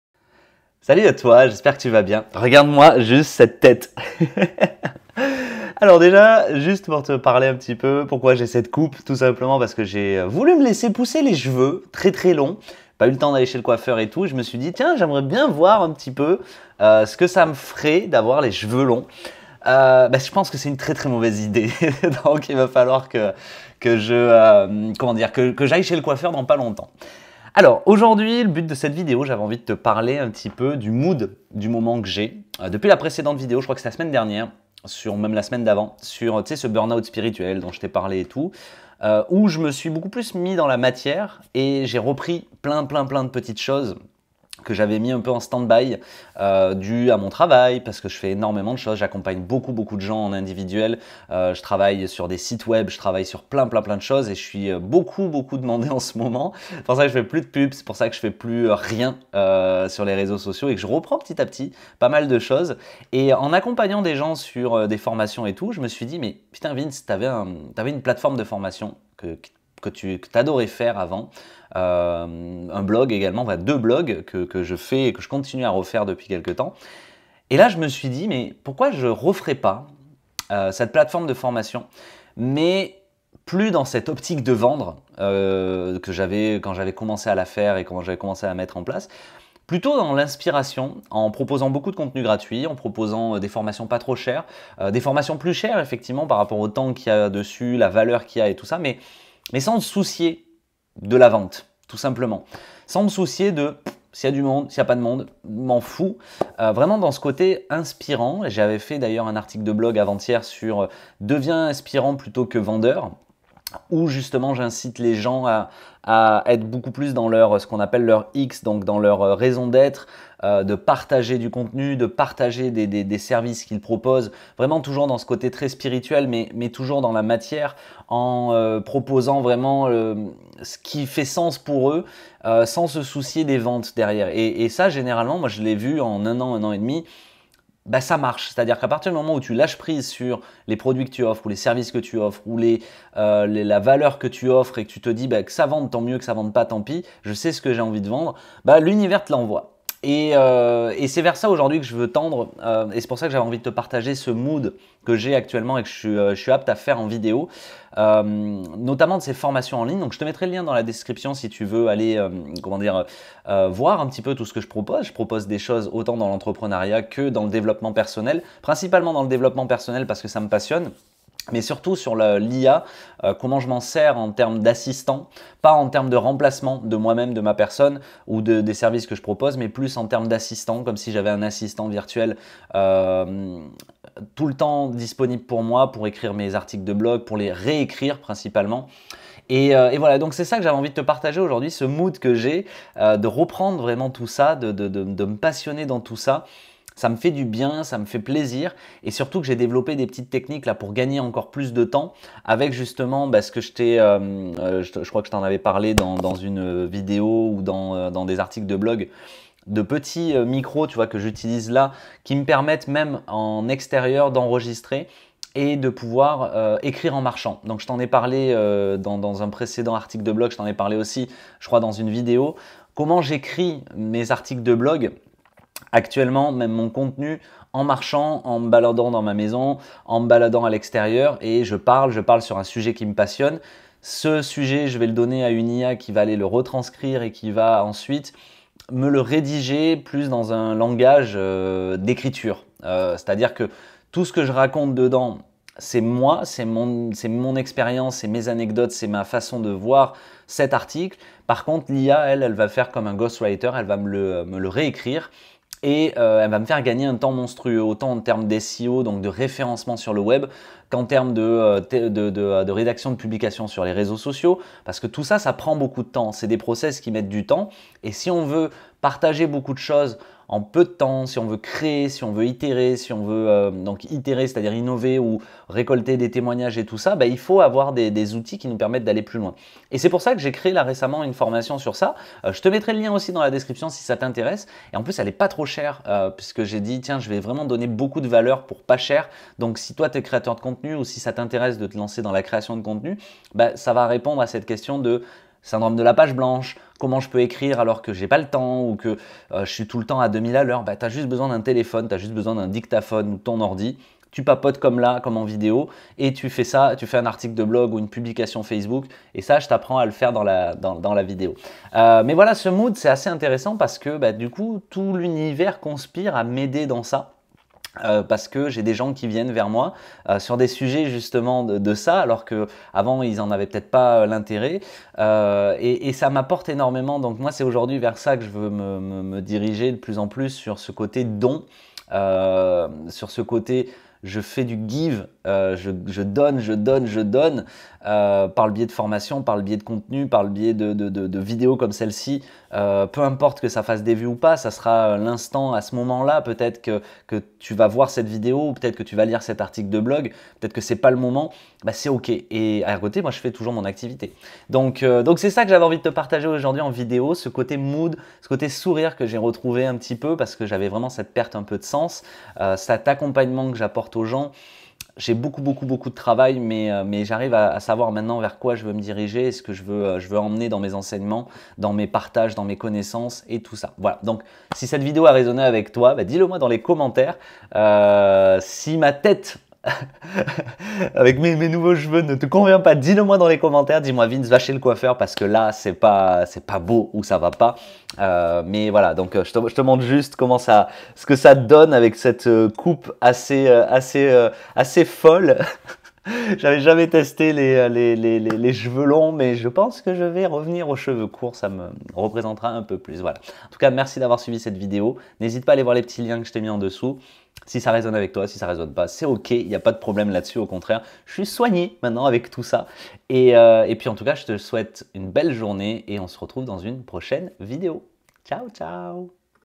SUPER MOOD et GRATITUDE ✨ : Devenir Inspirant grâce à l'IA ! �
Hello ! 👋 J'espère que tu vas bien ? 😊

Aujourd'hui, je partage avec toi mon super mood du moment qui me remplit de gratitude ! 🙏

Je me suis replongé dans la création de formations en ligne, mais cette fois avec une approche différente : devenir inspirant plutôt que de vendre. 💡

Je te raconte comment l'IA m'aide à créer du contenu et à booster ma productivité, même avec un emploi du temps chargé. 🚀 Découvre les outils que j'utilise et comment ils me permettent de gagner un temps précieux. ⏱️

👉 Si tu veux en savoir plus sur mes formations, le lien est juste

Dis-moi ce que tu penses de cette vidéo dans les commentaires ! 👇
On se retrouve bientôt sur la chaîne. 😘

Chapitres :
00:00 Intro (et ma nouvelle coupe de cheveux 😅)
00:30 Mon "mood" actuel : le don et l'inspiration ✨
01:04 La plateforme de formation que j'ai créée et mes 2 blogs 💻
01:43 L'IA comme assistant pour les créateurs de contenu 🤖
02:53 Gagner du temps avec l'IA : écrire et enregistrer en marchant 🚶
03:39 Ma formation sur l'IA pour les créateurs de contenu 💡
04:40 Le "give" : donner de la valeur sans se soucier des ventes 🎁
05:13 L'univers conspire à m'aider 🙏
05:55 Conclusion et appel à l'action �